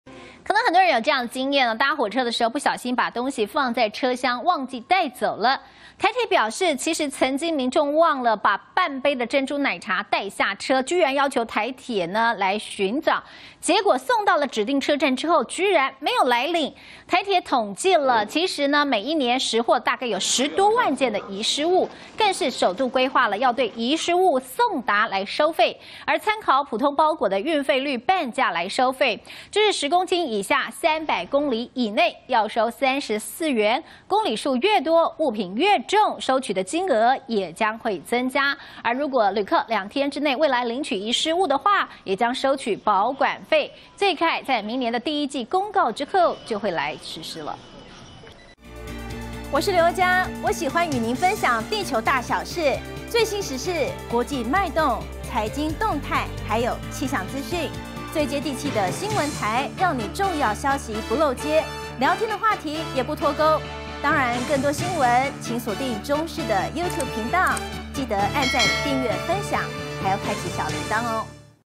忘拿珍奶求助台鐵卻未領 "失物運送"明年將收費│中視新聞 20221224
你曾經搭火車把物品放在車廂忘記帶走嗎?台鐵表示，曾經民眾忘了，把半杯珍珠奶茶帶下車，要求台鐵尋獲，結果送到指定車站，卻沒來領台鐵統計，每年拾獲十多萬件遺失物，也首度規劃，要對遺失物送達收費，參考普通包裹運費率半價收費，10公斤以下、300公里以內要收34元，公里數越多，物品越重，收的金額也將增加，若旅客2天內未來領取遺失物，也將收取保管費，最快明年第一季公告後實施。